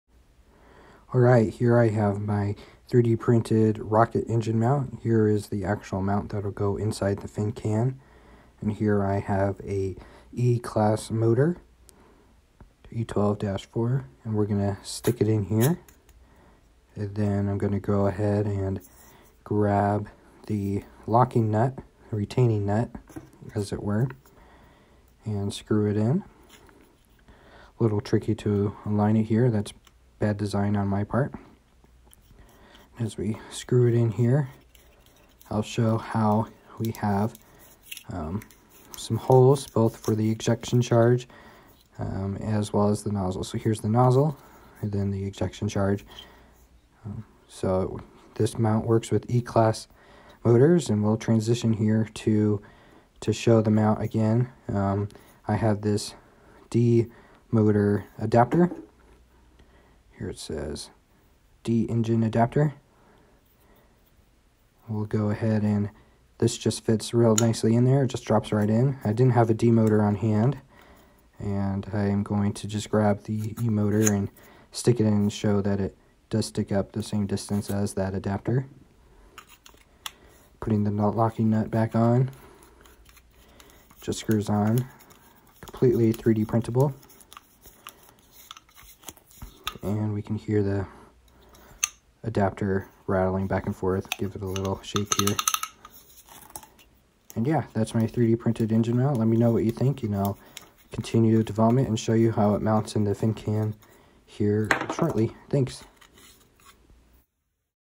3D Printed Rocket Motor Mount
This is a motor mount that I designed and printed to fit my upcoming rocket that I am designing. This mount is for E sized motors and will also fit D sized motors as well.